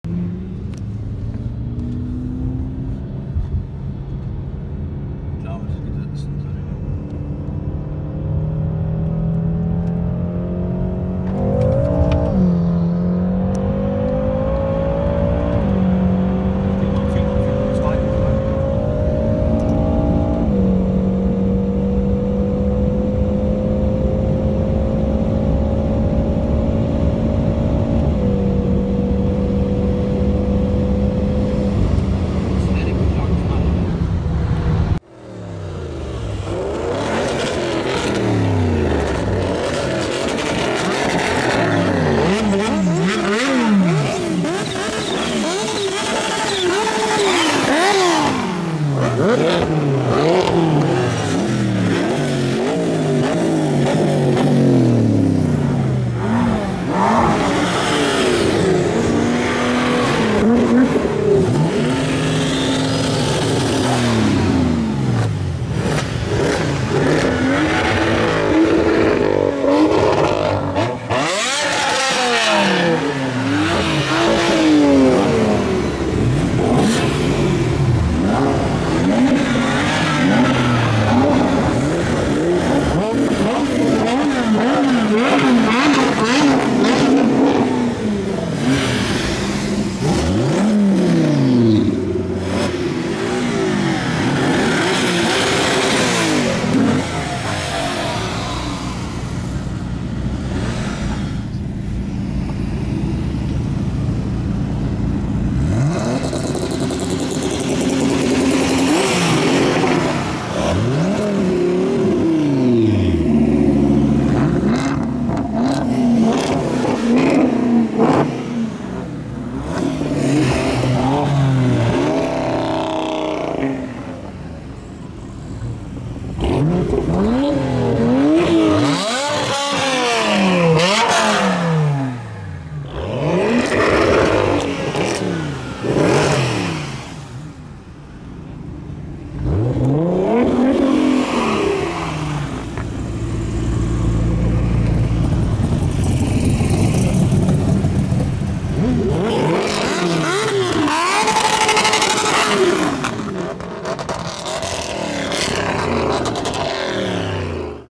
HC Performance BMW M6 Coupe Competition Package vs Porsche 991 Turbo S Mk I 560 HP (from 80 km/h)
BMW M6 tuned with ECU, downpipes and KN "drop" in filter. HC Performance